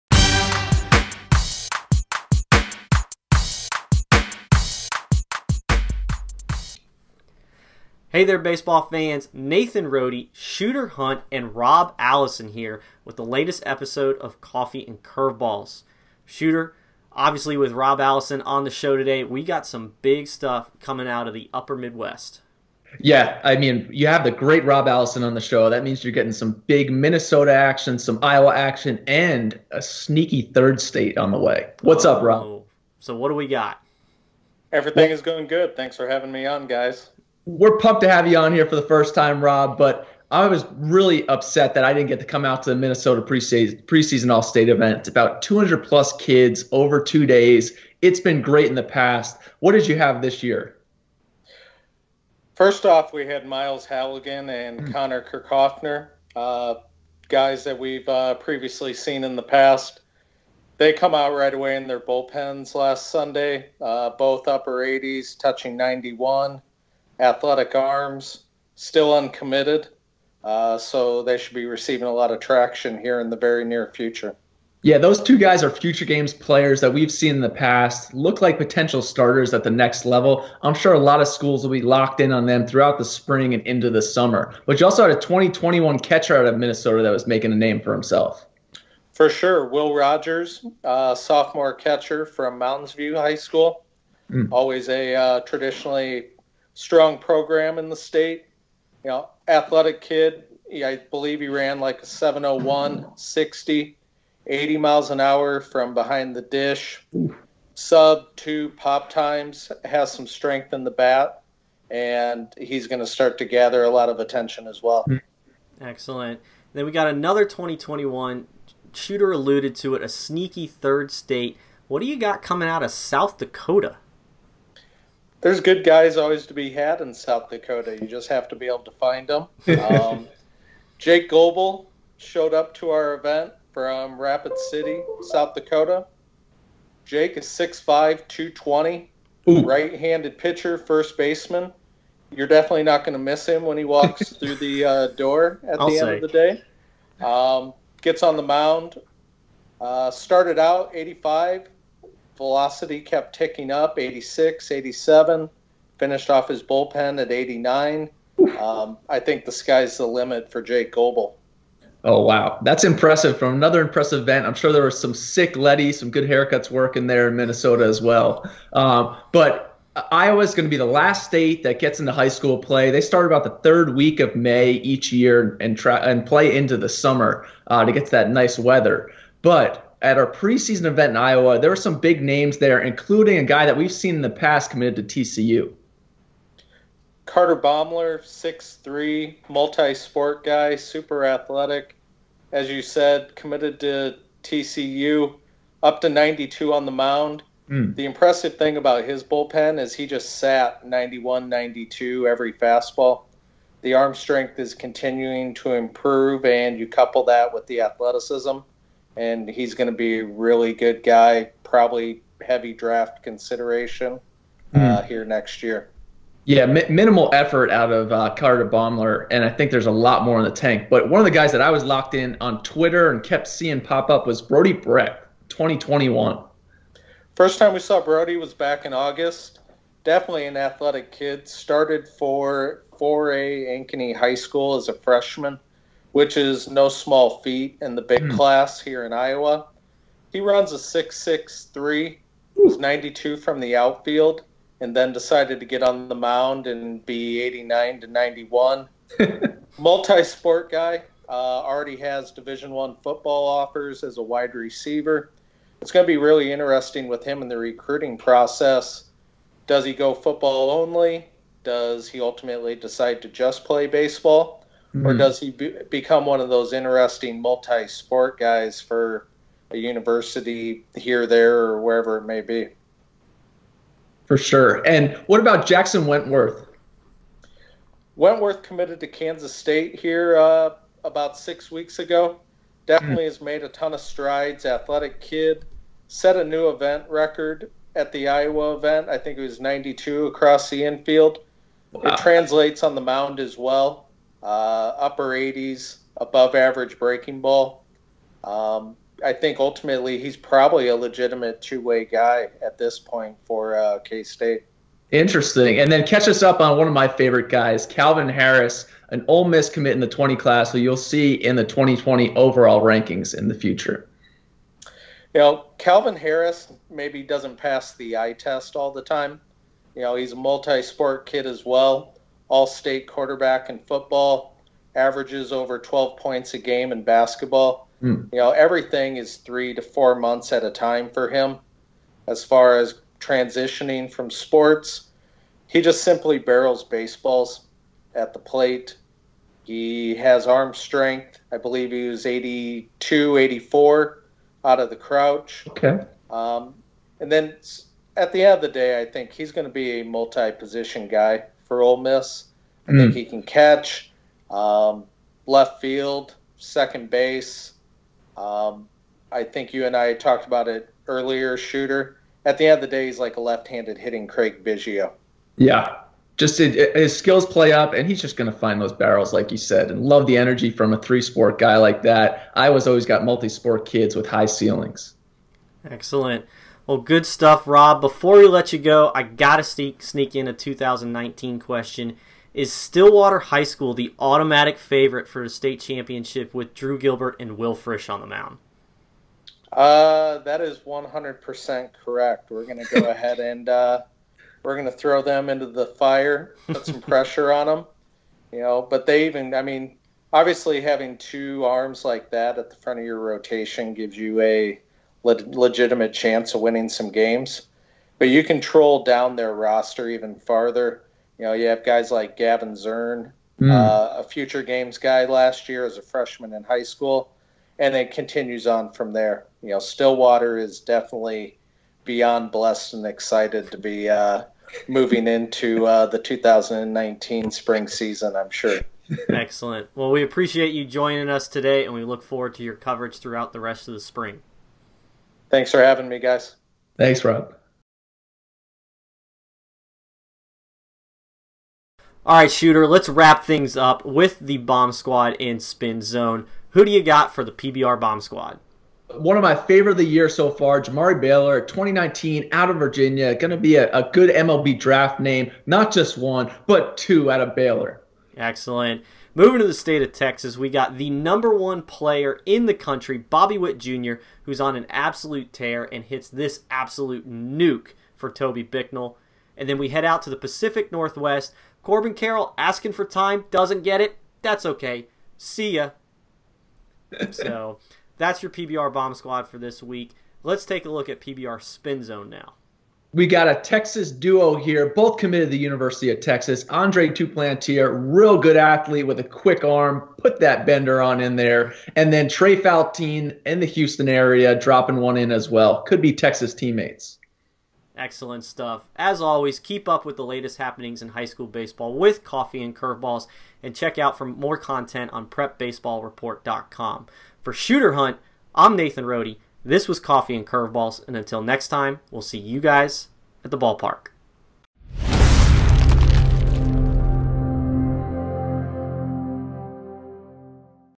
Coffee & Curveballs: March 26th
Rob Allison joins the guys to talk about players from his recent events in Minnesota and Iowa.